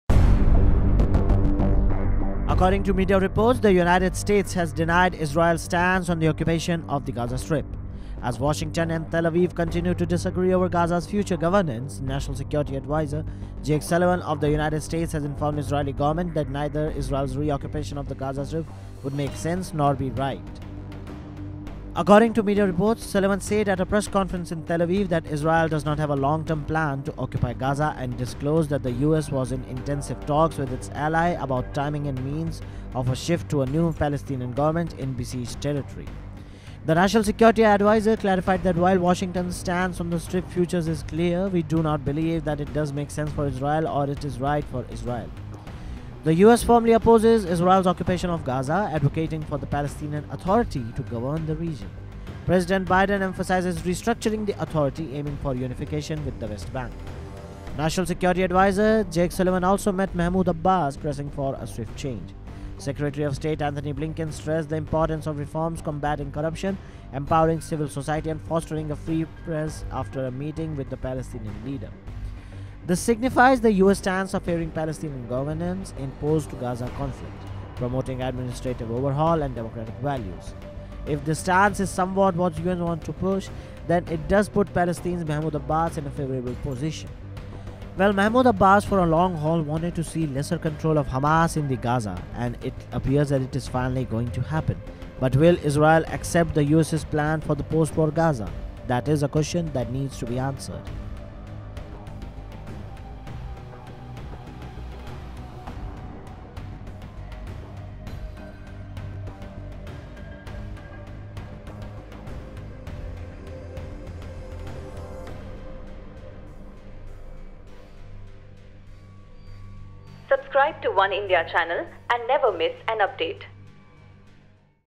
U.S Denies Israel’s Plan Of Gaza Occupation. Instead, It Proposes New Administrative Strategy
Media reports cite the US denying Israel's Gaza occupation plan. National Security Adviser Jake Sullivan opposes Israel's reoccupation, deeming it illogical. The US aims for a new Palestinian administration in Gaza, urging changes in governance, emphasizing reforms and support for Palestinian Authority's control post-conflict.